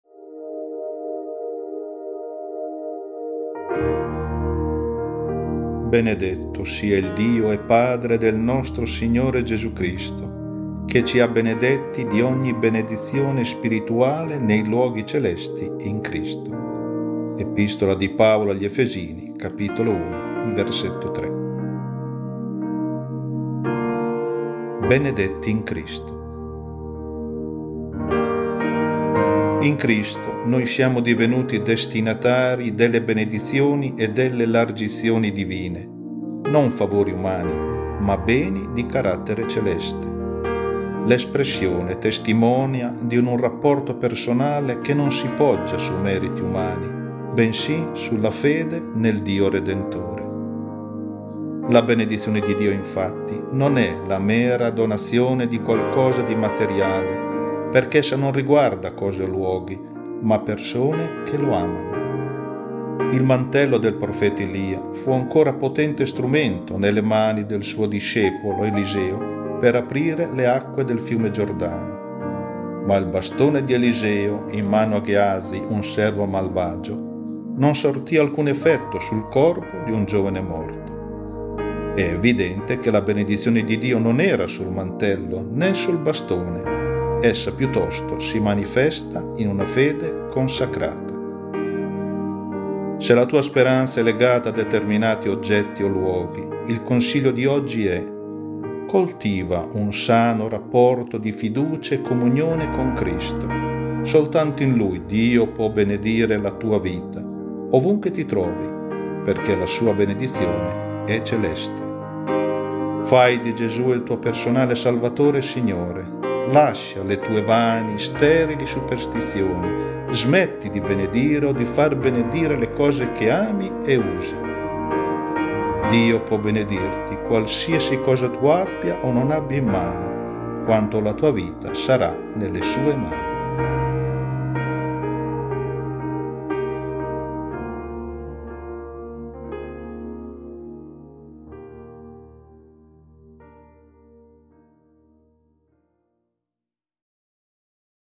"Benedetti in Cristo" - Meditazione Efesini 1:3
“Benedetto sia il Dio e Padre del nostro Signore Gesù Cristo, che ci ha benedetti di ogni benedizione spirituale nei luoghi celesti in Cristo.” Efesini 1:3

BENEDETTI IN CRISTO

In Cristo noi siamo divenuti destinatari delle benedizioni e delle elargizioni divine: non tanto favori umani, ma quanto beni di carattere celeste. L’espressione testimonia di un rapporto personale che non si poggia su meriti umani, bensì sulla fede del Redentore. La benedizione di Dio, infatti, non è la mera elargizione di qualche dono materiale, perché essa non riguarda cose o luoghi, ma persone che Lo amano. Il mantello del profeta Elia fu ancora potente strumento, nelle mani del suo discepolo Eliseo, per aprire le acque del fiume Giordano, ma il bastone di Eliseo in mano a Gheazi, un servo malvagio, non sortì alcun effetto sul corpo di un giovane Morto. È evidente che la benedizione di Dio non era su un mantello, ne su un bastone, ma si manifesta nella fede consacrata.
La tua speranza è legata a determinati oggetti e luoghi? Coltiva un sano rapporto di fiducia comunione con Cristo. Soltanto in Lui Dio può benedire la tua vita, ovunque ti trovi, perché la Sua benedizione è celeste. Fai di Gesù il tuo personale Salvatore e Signore. Lascia le tue vane, sterili superstizioni, smetti di benedire o far benedire le cose che ami e usi. Dio potrà benedirti, qualsiasi cosa tu abbia o non abbia in mano, quando la tua vita sarà nelle Sue mani!